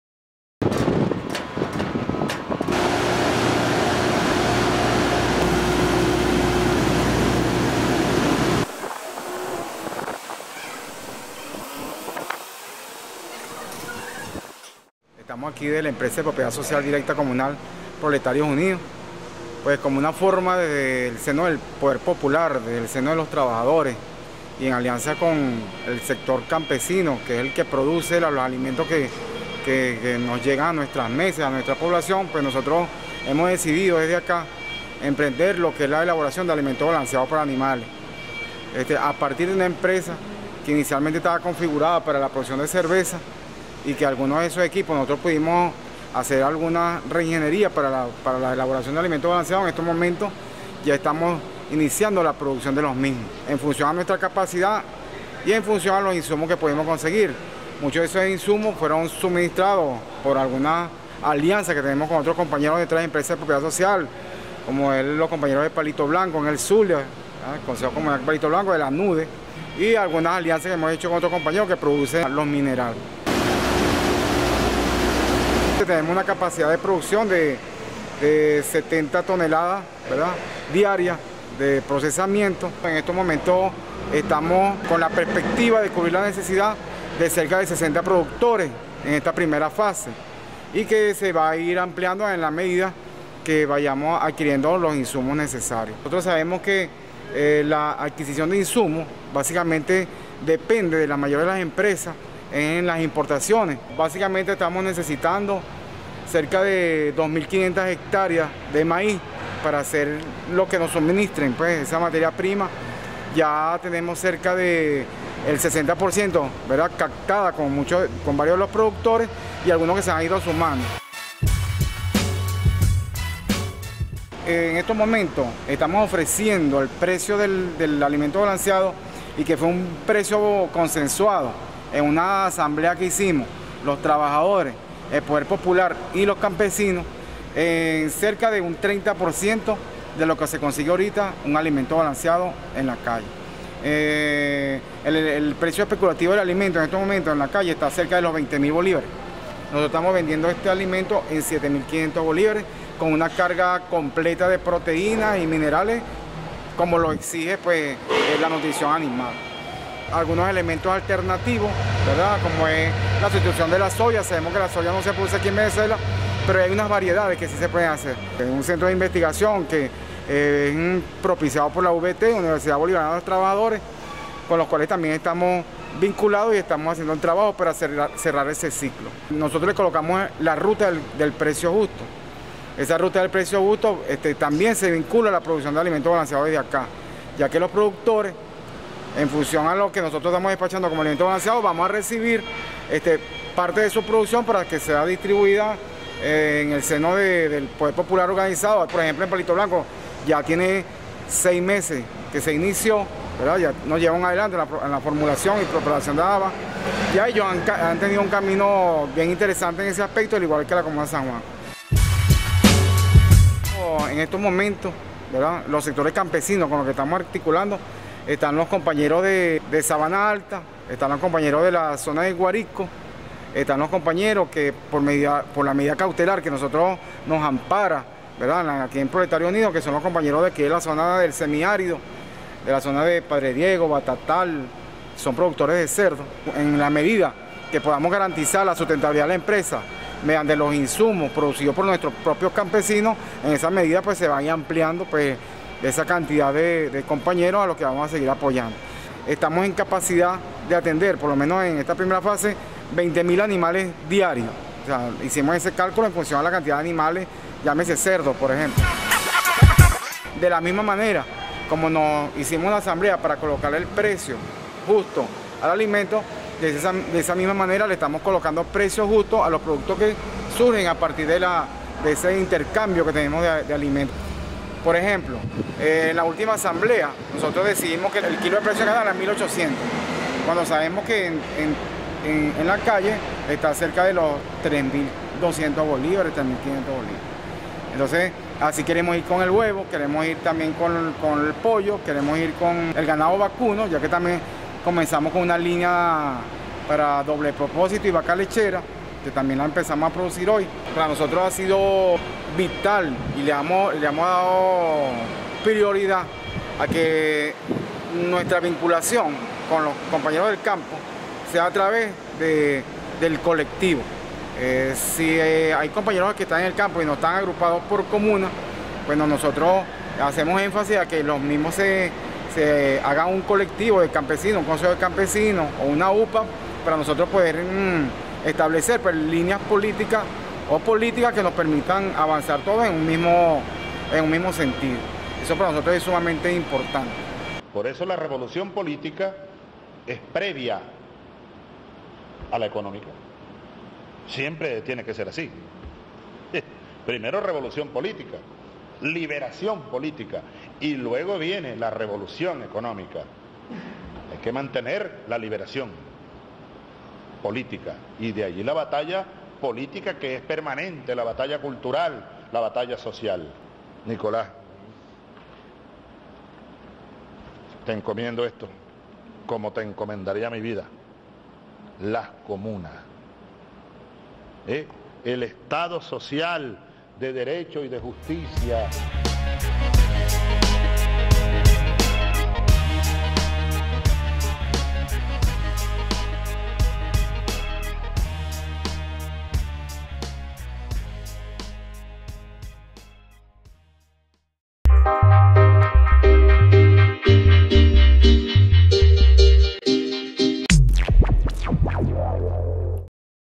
EPSDCproletariosunidosylacomunajosepiotamayoproducenalimentos(abas)
la comuna jose pio tamayo y la epsdc proletarios unidos junto al consejo campesino argimiro gabaldon con varias comunas del país y movimientos sociales activan la producción de alimentos para animales (abas) en barquisimeto estado lara impulsando la economía comunal.chavez vive la lucha sigue... comuna o nada.